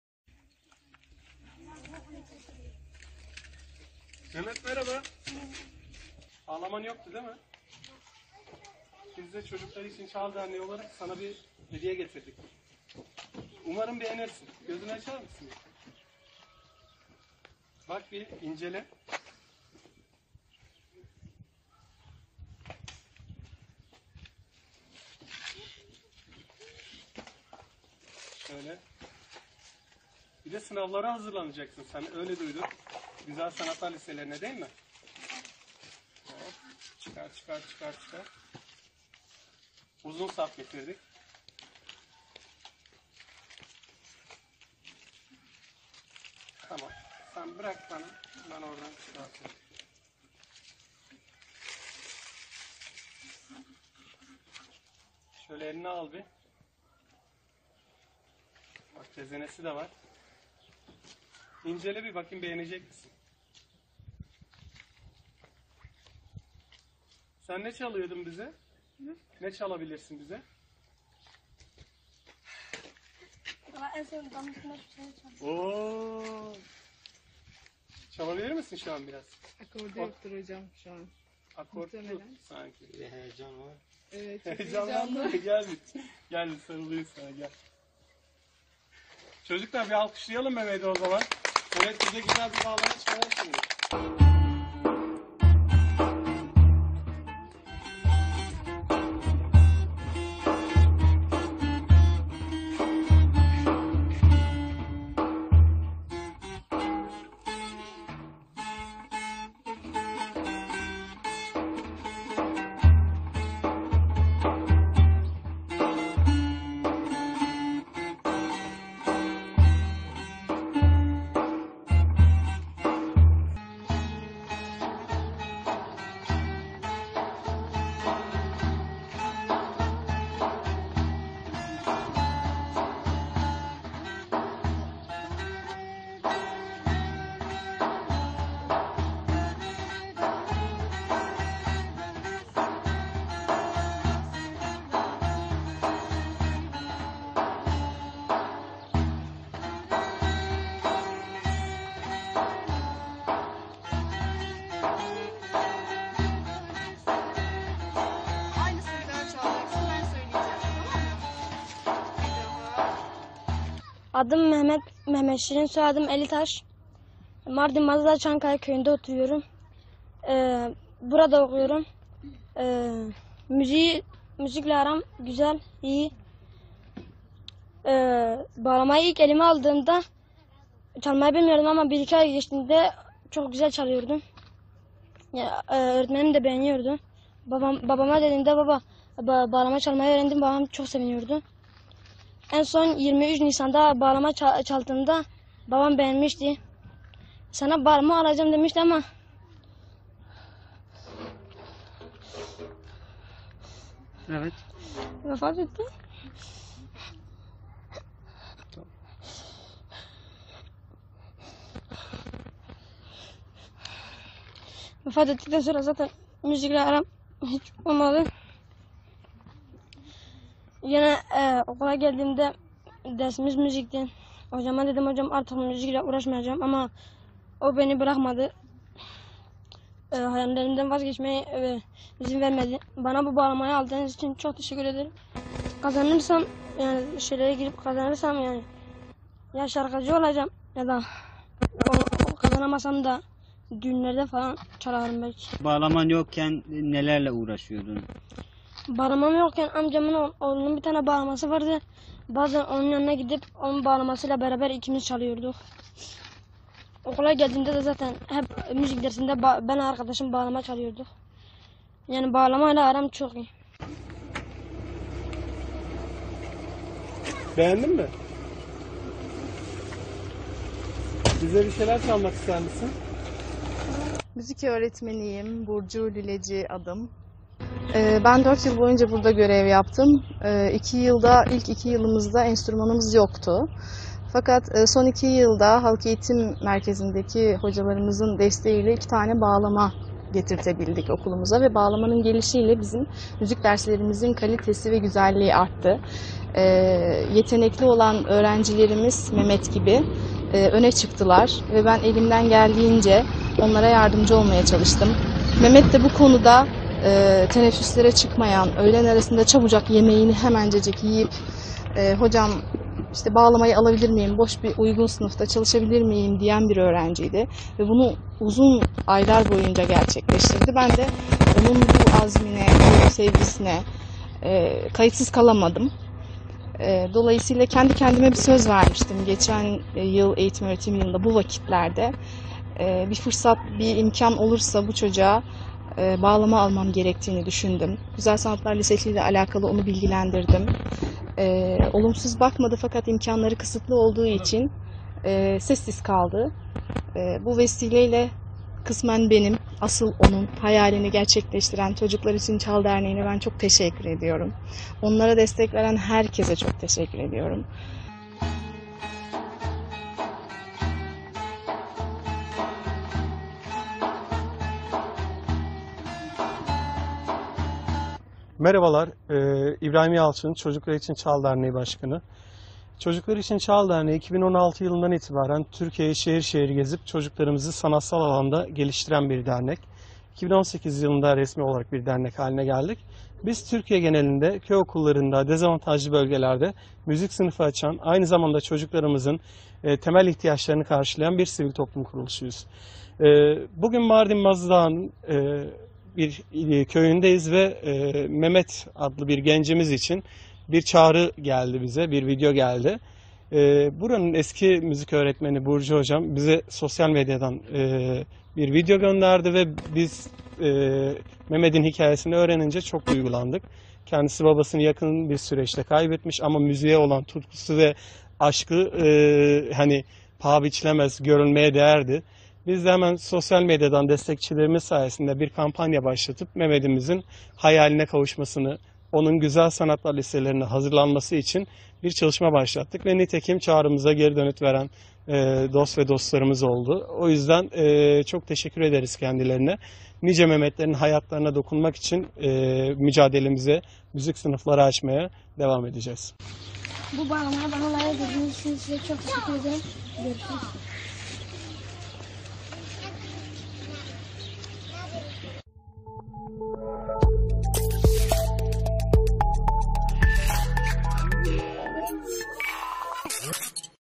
Babası söz verip alamadan hayatını kaybetti, 6 ay sonra onun için çalmaya başladı
Mardin'de 8'inci sınıf öğrencisi Mehmet Şirin Elitaş'ın babası, oğluna bağlama alacağı sözünü verdi. Babasının, hayatını kaybetmesi ile dünyası başına yıkılan genç, çok sevdiği müzikle arasına mesafe koydu. Müzik öğretmeninin girişimleri ile kendi bağlamasına kavuşan Elitaş, bağlamayı hayatını kaybeden babası için çalmaya başladı.

Mardin'in Mazıdağı ilçesine bağlı kırsal Çankaya Mahallesinde yaşayıp, taşımalı eğitim ile Karalanı Mahallesinde öğrenim gören 8'inci sınıf öğrencisi Mehmet Şirin Elitaş, Güzel Sanatlar Lisesinde okuma hayali kurmaya başladı. Kendi bağlaması olmayan ve yakınlarından ödünç alarak öğrenmeye çalışan Elitaş'a babası, bağlama alacağını söyledi. Bir süre sonra babası hayatını kaybeden Elitaş'ın dünyası başına yıkıldı. Babasını kaybetmesinin üzüntüsünü yaşayan Mehmet Şirin, müzik öğretmenine bağlama çalmayı bırakmak istediğini söyledi. Hayallerini bırakmasına izin vermeyen müzik öğretmeni Burcu Lüleci'nin destekleriyle Güzel Sanatlar Lisesine hayalini bırakmayan Elitaş, bağlama çalmaya tekrar başlamak istedi. Ancak bağlaması olmayan Elitaş, 6 ay boyunca bağlama çalmadı. Çocuklar İçin Çal Derneğinin sosyal medyada başlattığı kampanya ile hayaline kavuşan Elitaş, çok sevdiği bağlamasına kavuştu. Her türlü zorluğa rağmen hayalini bırakmayan Elitaş, hayatını kaybeden babasının anısına Güzel Sanatlar Lisesi sınavını kazanmak için yeniden kollarını sıvadı.

"Babam bağlama çalmamı çok beğenmişti"

Babasının bağlama çalışını çok beğendiğini belirten Mehmet Şirin Elitaş, bundan sonra hep babası için çalacağını söyledi. Elitaş, "Müzikle aram çok çok iyi, bağlamayı ilk olarak elime aldığımda çalmayı bilmiyordum ama 1-2 ay geçtiğinde çok güzel çalıyordum, müzik öğretmenim de beğeniyordu. Babama bağlama çalmayı öğrendiğimi söylediğimde çok sevinmişti. En son 23 Nisan Ulusal Egemenlik ve Çocuk Bayramında bağlama çaldığımda babam çok beğenmişti, 'sana bağlama alacağım' demişti ama vefat etti. Babam vefat ettikten sonra da zaten müzikle hiç aram olmadı. Okula geldiğimde müzik öğretmenime artık müzikle uğraşmayacağımı söyledim ama müzik öğretmenim hayallerimi bırakmama izin vermedi ve onun destekleriyle bağlama sahibi oldum. Güzel Sanatlar Lisesini kazanırsam şarkıcı olmayı düşünüyorum, kazanamazsam da düğünlerde çalarım belki. Bağlamam yokken amca oğlumun bir bağlaması vardı, bazen onun yanına gidip, onun bağlamasıyla beraber ikimiz çalıyorduk. Hayallerime sımsıkı çalacağım ve hep babam için çalacağım" dedi.

"Mehmet'in azmine kayıtsız kalamadım"

Kararlılık ve azimle öğrenimini sürdüren öğrencisinin durumuna kayıtsız kalamadığını ifade eden müzik öğretmeni Burcu Lületaş ise "Dört yıl boyunca Karalanı köyünde görev yaptm, ilk 2 yılımızda enstrümanımız yoktu, fakat son 2 yılda Halk Eğitim Merkezindeki hocalarımızın desteğiyle 2 tane bağlama okulumuza getirtebildik ve bağlamaların gelişiyle bizim müzik derslerimizin kalitesi ve güzelliği arttı. Mehmet Şirin gibi yetenekli olan öğrencilerimiz öne çıktılar ve ben elimden geldiğince onlara yardımcı olmaya çalıştım. Mehmet de bu konuda teneffüslere çıkamayan ve öğle aralarında çabucak yemeğini yiyip, 'Hocam bağlamayı alabilir miyim, uygun ve boş bir sınıfta çalışabilir miyim' diyen bir öğrenciydi. Bu azmini ve çabasını uzun aylar boyunca sürdürdü, ben de onun bu azim ve kararlılığına kayıtsız kalamadım. Dolayısıyla kendi kendime geçen yıl bir söz vermiştim, bir fırsat olursa bu çocuğumuza bağlama almam gerektiğini düşündüm, Güzel Sanatlar Lisesiyle alakalı onu bilgilendirdim. Olumsuz bakmadı fakat imkanları kısıtlı olduğu için sessiz kaldı. Çocuklar İçin Çal Derneğinin vesilesiyle kısmen benim kısmen de Mehmet Şirin'in hayalini gerçekleştirdik. Çocuklar İçin Çal Derneğine çok teşekkür ederim, onlara destek veren herkese çok teşekkür ediyorum" diye konuştu.

"Mehmet gibi çocuklarımızın hayallerini gerçekleştirmeye devam edeceğiz"

Köy okullarında müzik sınıfı açan Çocuklar İçin Çal Derneği Başkanı İbrahim Yalçın da dezavantajlı çocukların hayallerini gerçekleştirmeye devam edeceklerini söyledi. Yalçın, "Çocuklar İçin Çal Deneği olarak, 2016 yılında beri Türkiye'yi şehir şehir gezip, çocuklarımızın sanatsal alanda geliştiren bir dernek olarak, 2018 yılında resmi olarak bir dernek haline geldikten sonra Türkiye genelinde köy okulları ve dezavantajlı bölgelerde müzik sınıfı açıp, aynı zamanda çocuklarımızın temel ihtiyaçlarını gideriyoruz. Bugün Mardin'in Mazıdağı ilçesine bağlı Karalanı Mahallesinde Mehmet Şirin kardeşimiz için bize buradaki eski müzik öğretmeni tarafından sosyal medyadan bir video geldi. Biz Mehmet'in durumunu öğrenince çok duygulandık. Kendisi babasını yakın bir süreçte kaybetmiş ama müziğe olan tutkusu ve aşkı paha biçilemez, görülmeye değerdi. Biz hemen sosyal medyadan destekçilerimiz sayesinde bir kampanya başlatıp Mehmet'imizin hayaline kavuşmasını ve güzel sanatlar lisesine hazırlanması için bir çalışma başlattık.
Serhat Abi
Özgür Kaya